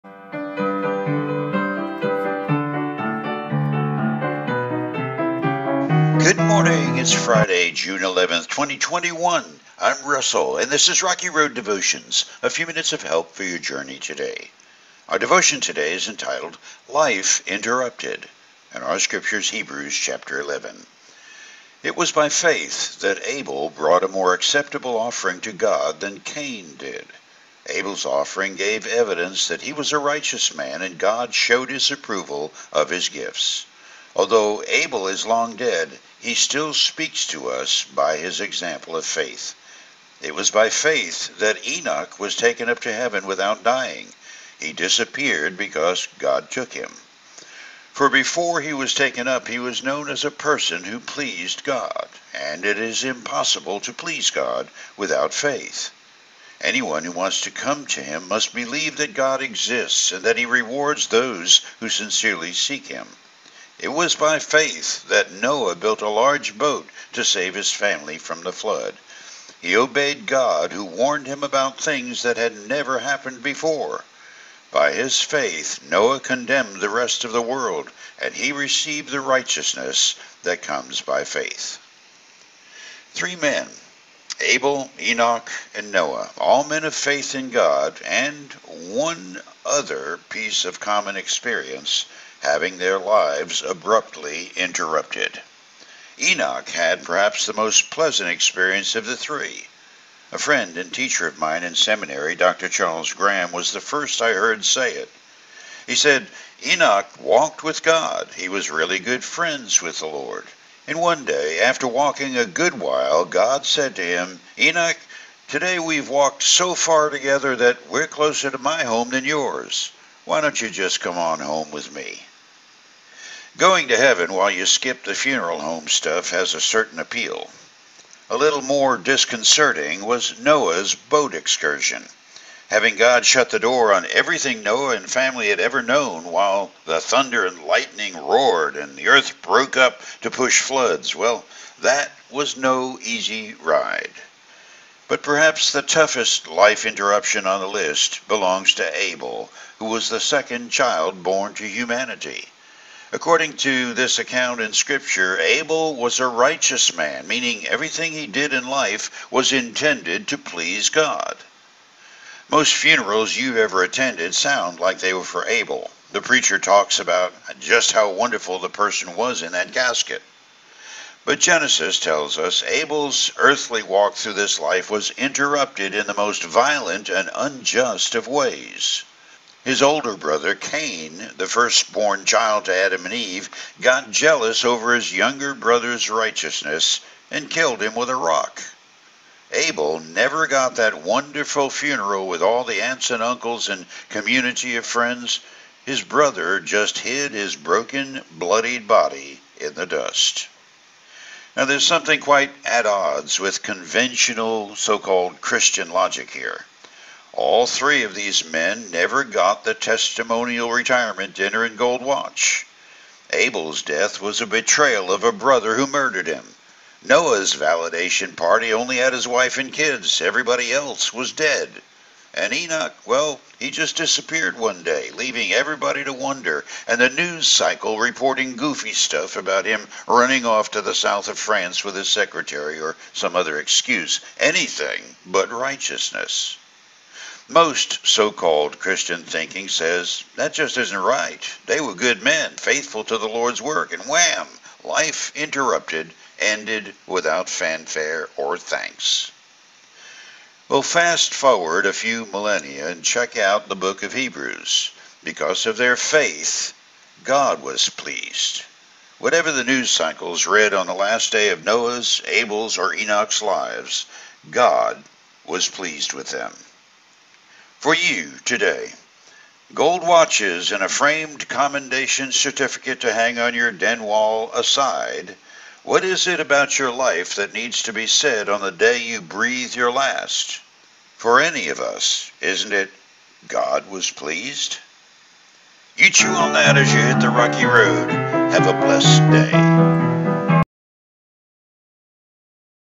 6 11 21 Life Interrupted
Three men, Abel, Enoch, and Noah – all men of faith in God, and one other piece of common experience – having their lives abruptly interrupted.  Enoch had, perhaps, the most pleasant experience of the three.